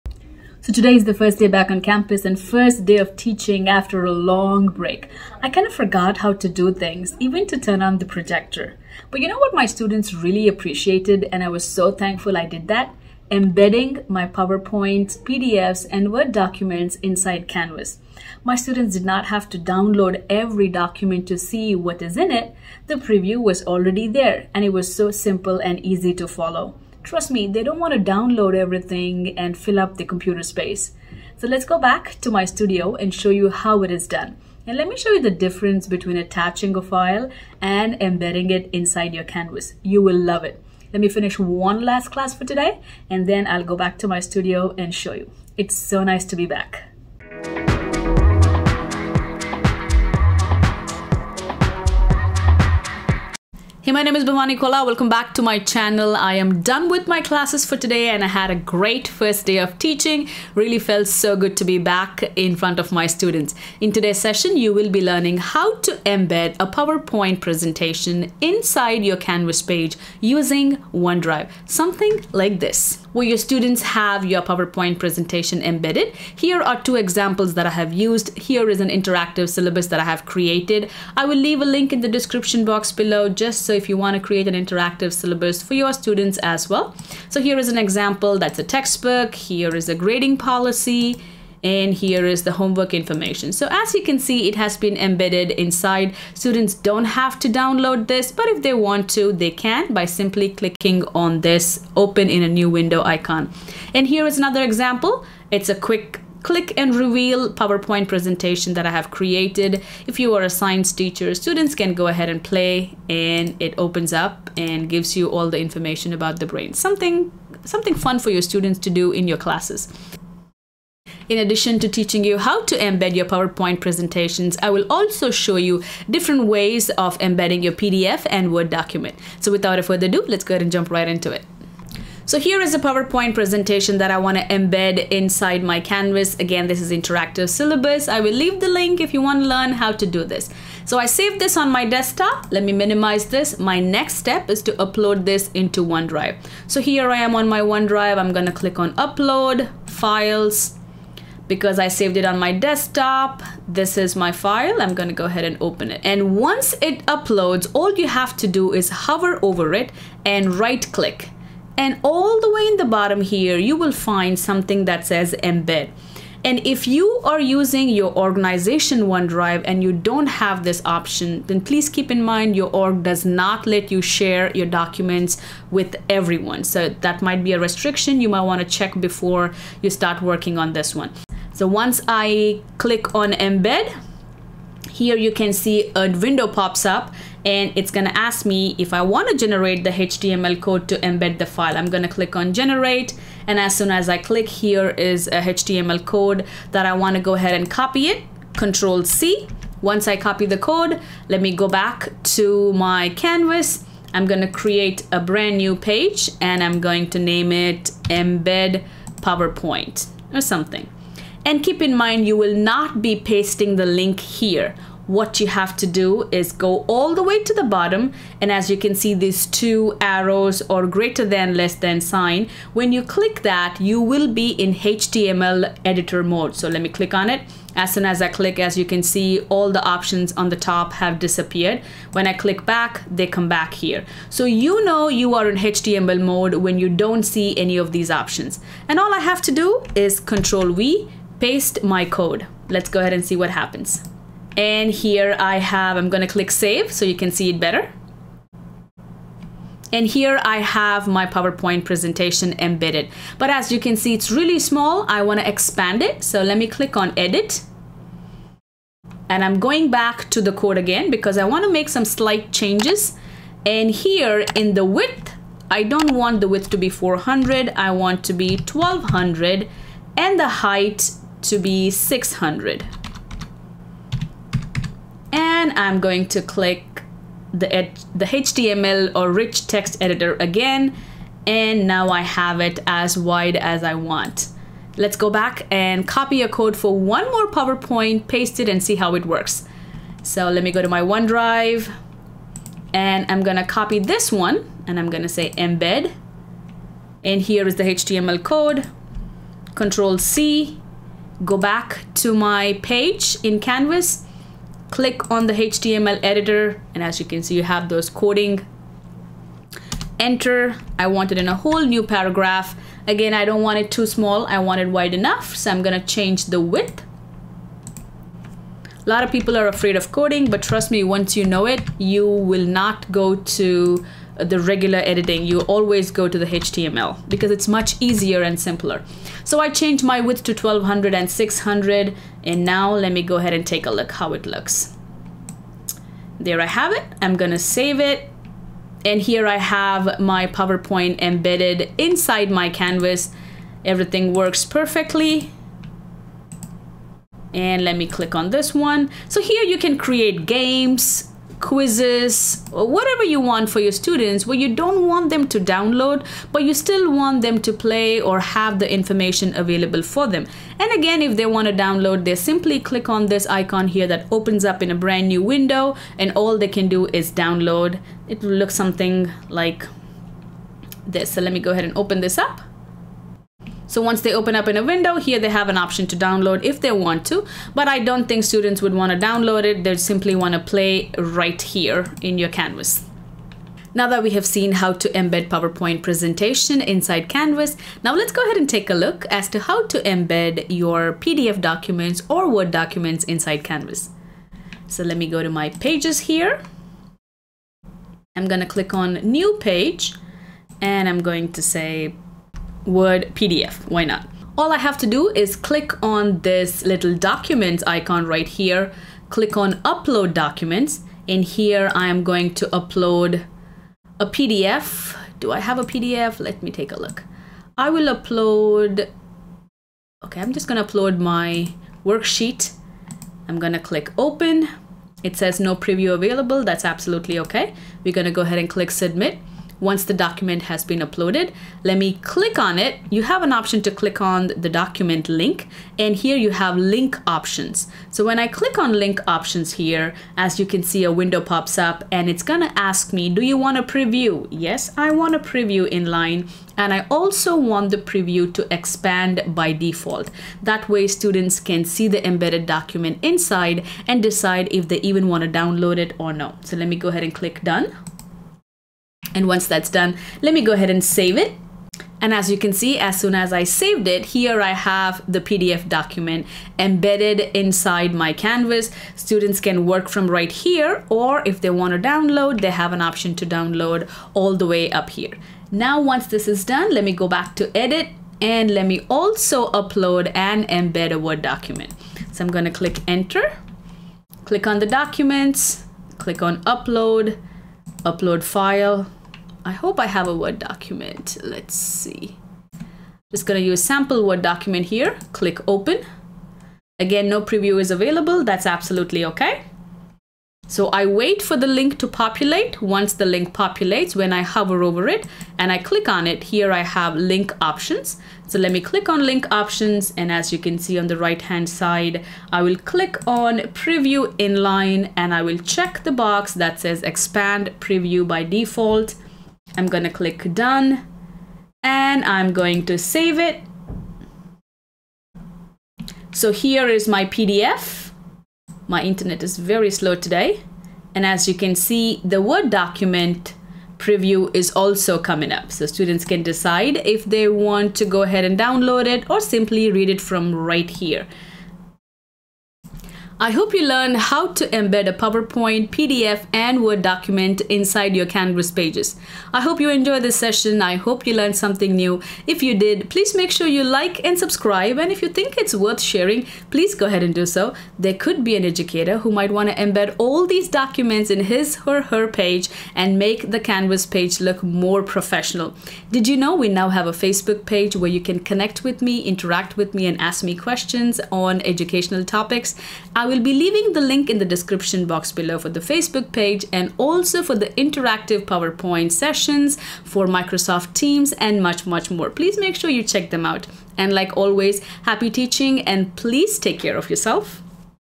How to embed PPT in Canvas
In this video you will learn how to embed PPT into Canvas using OneDrive.
I will show you how to edit the HTML code inside canvas to customize your PPT slideshow.
I will also demonstrate how to embed PDF and Word into Canvas
Please note that some organizations do not allow embedding. If that is the case open a free One drive account and the follow the steps shown in the video.
THIS IS AN ALTERNATIVE WAY TO EMBED PPT IN CANVAS FOR ORGANIZATIONS WHO DO NOT ALLOW EMBEDDING.

If your organization allows embedding, then go to PPT online version and Click on Files Share---Embed and copy the embed code.

0:00 Intro
1:12 Demo
2:26 How to upload your PPT to Onedrive
2:38 How to get your embed code
3:24 How to embed in Canvas page using HTML editor
4:25 How to edit the HTML code to change the width and height of PPT
6:40 How to Embed your PDF files
8:01 How to Embed your word document

Amazon links to the products I use in the video